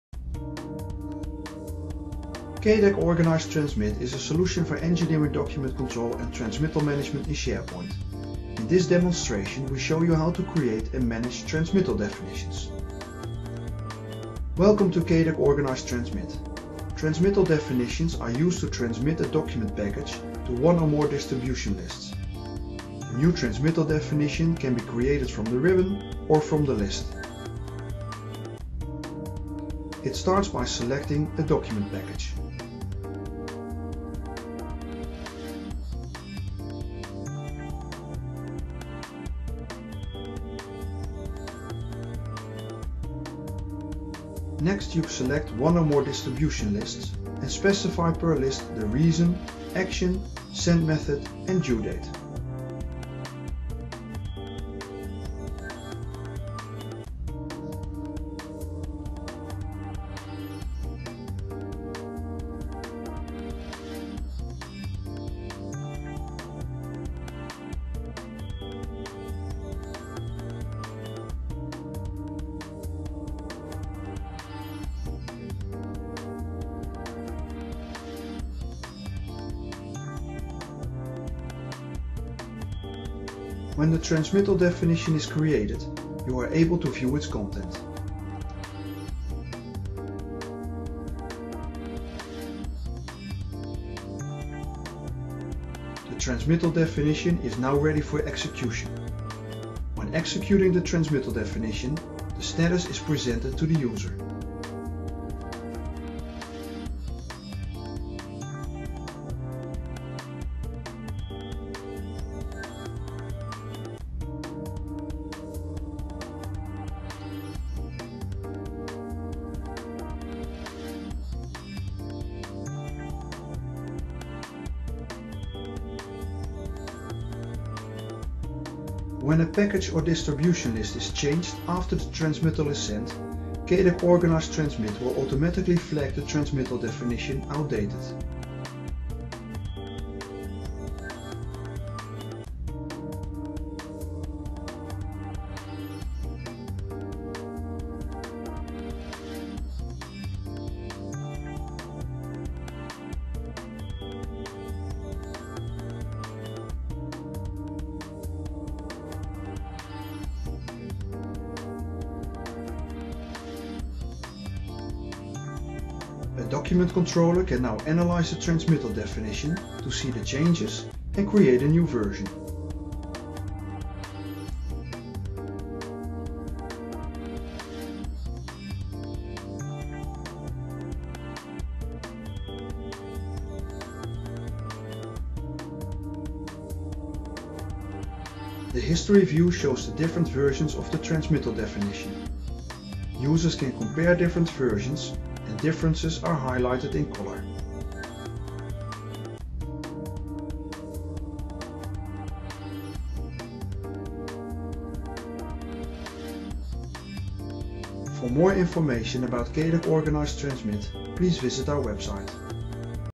Cadac Organice Transmit - Creating and Managing Transmittal Definitions
Creating and managing transmittal definitions for transmittal management in SharePoint with Cadac Organice Transmit